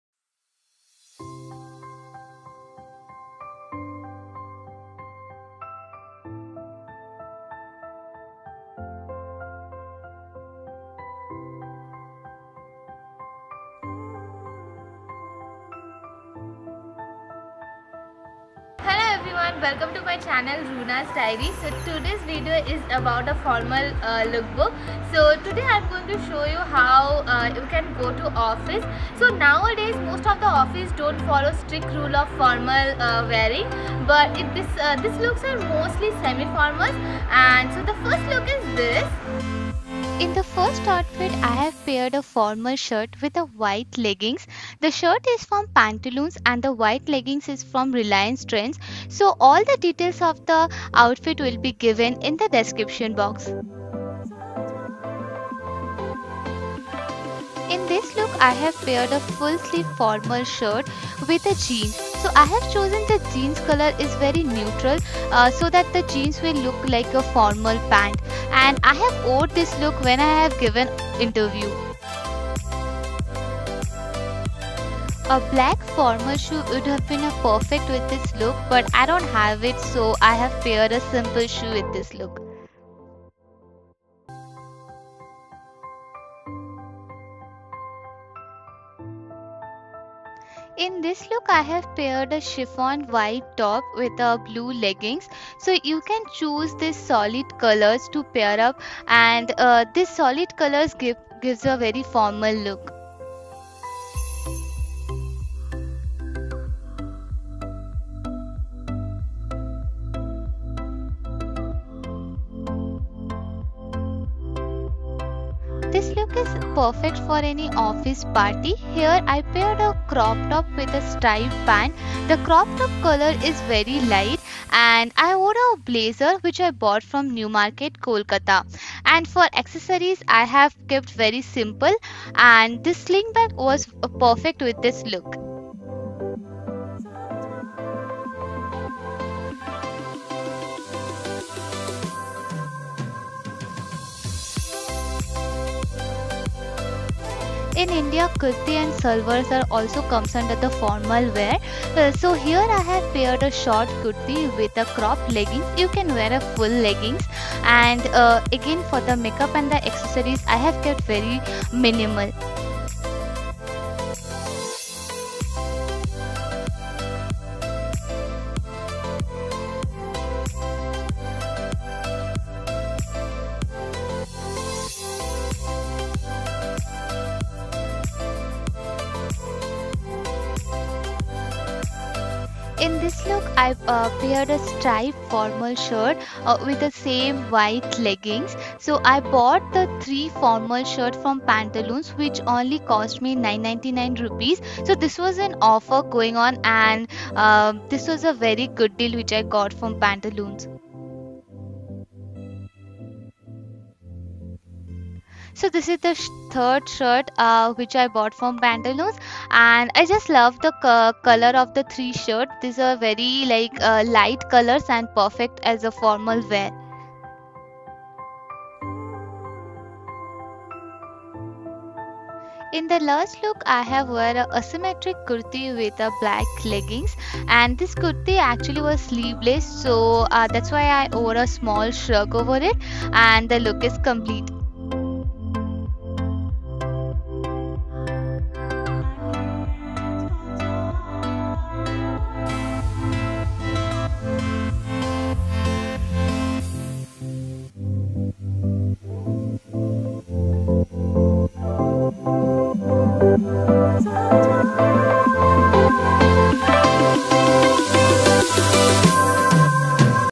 Office Wear Guide for Women! What to wear to an interview | Runa’s Diary
OFFICE LOOKBOOK | Professional Outfit Ideas!

WORK OUTFIT IDEAS | Professional Work Office Wear Lookbook

Hello Everyone,

Here's a useful video for all the office going girls and ladies. It's very important to wear stylish yet comfortable clothes in office. So here are few tips!! It's a concise Office Wear/ Formal Wear Video. It's a complete Office Lookbook. Right from Pants, Shirts, Dresses, Bags and Accessories to Blazers! It covers all :)

Look 1 -
Shirt : Pantaloons
Leggings : Reliance Trends

Look 2 -
Pink Shirt and Jeans : Pantaloons
Shoe : Commercial Streets

Look 3 -
Top : Myntra
Blue Jeggings: Pantaloons

Look 4 -
Crop Top : New Market
Striped Pants : Myntra
Sling Bag : New Market
Blazer : New Market

Look 5 -
Kurti : Pantaloons
Leggings : Go Colors

Look 6 & 7 -
Shirt :  Pantaloons

Look 8 –
Kurti : Brand Factory
Leggings : Go Colors
Shrug : Local Shop
Shoe : Khadims